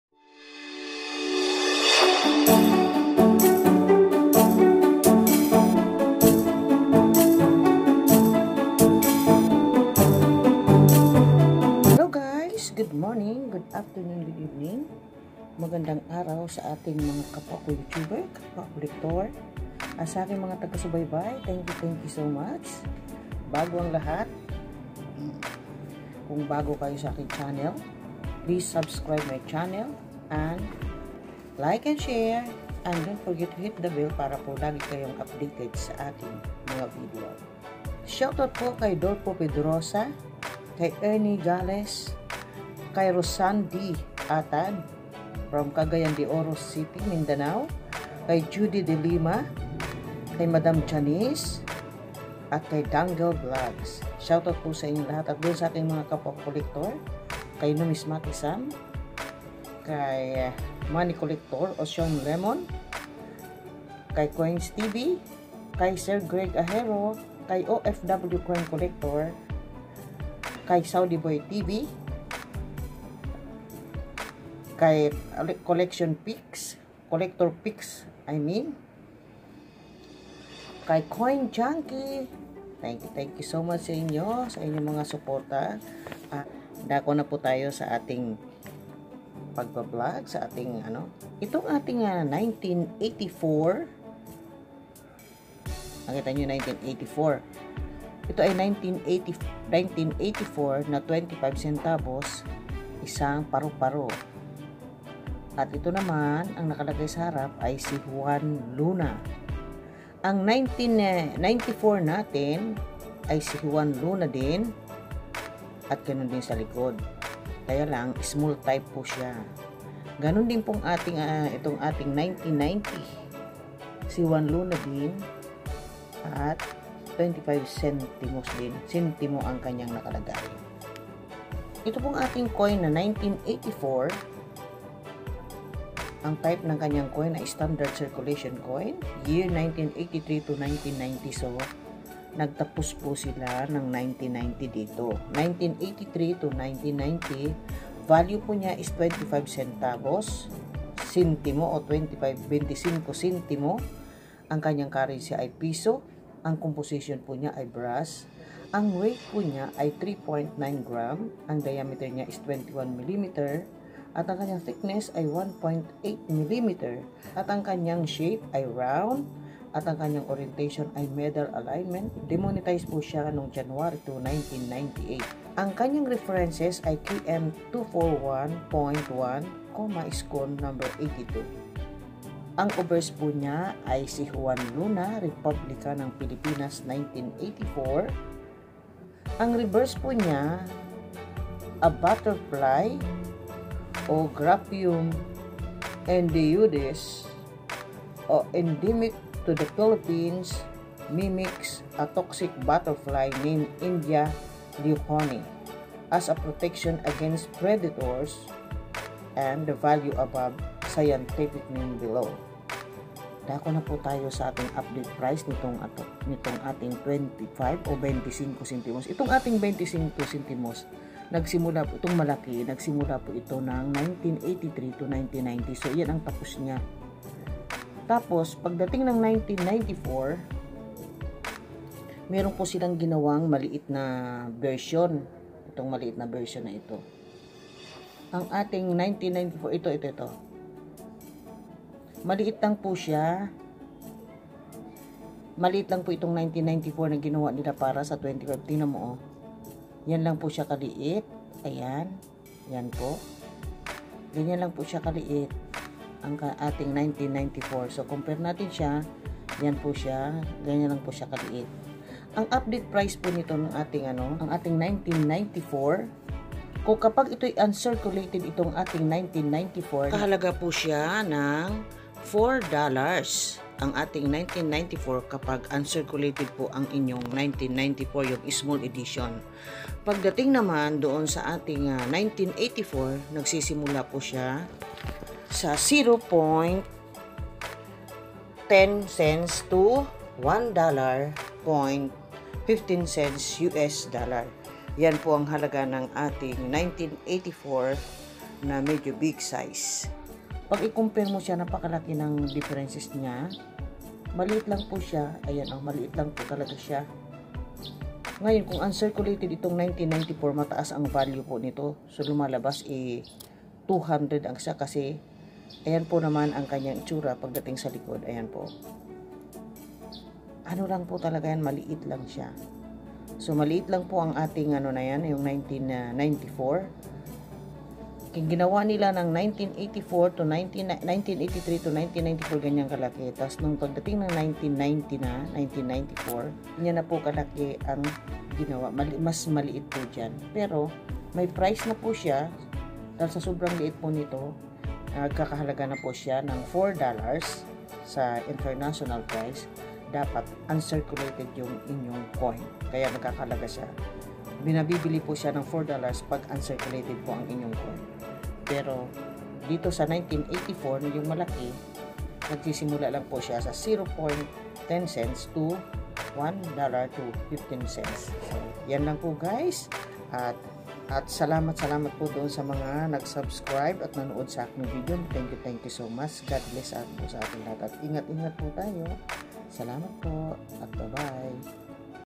25 sentimos 1984-1994 Alin ang 200 Ang halaga
Obverse:
Bust of Juan Luna facing right
REPUBLIKA NG PILIPINAS

Reverse:
A butterfly (Graphium idaeoides)
Endemic to the Philippines, mimics
A toxic butterfly named Idea leuconoe
As a protection against predators,
And the value above, scientific name